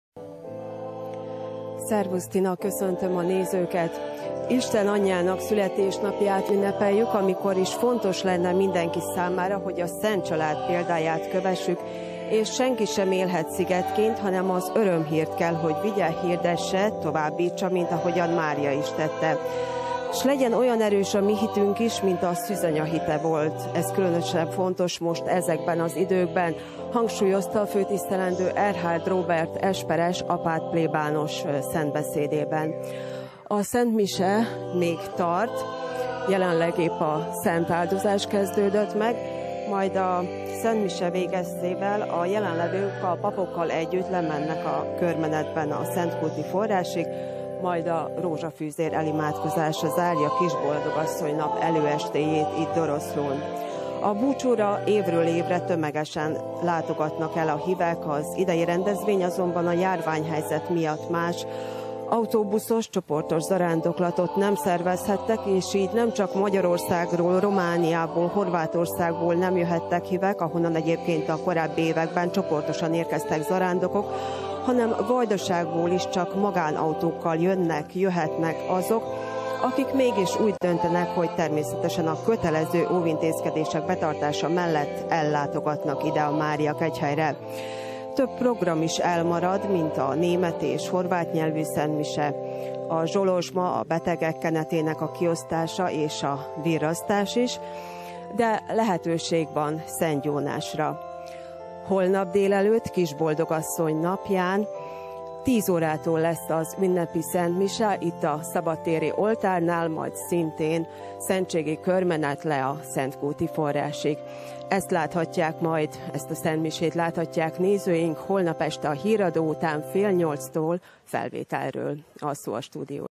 A Szentkúti Búcsú előestéjén
DOROSZLÓ - A járványhelyzet miatt a doroszlói Mária, Keresztények Segítsége Egyházmegyei Kegyhelyen ünnepi szentmisét tartanak ma és holnap, a virrasztás azonban elmarad. Azok, akik ellátogattak Doroszlóra a főbúcsú előestéjén, 18 órától részt vehettek a szentmisén és a körmeneten. A helyszínen van Ökrös Edit kolléganőnk. Milyen üzenete van az idén a Szentkúti Búcsúnak?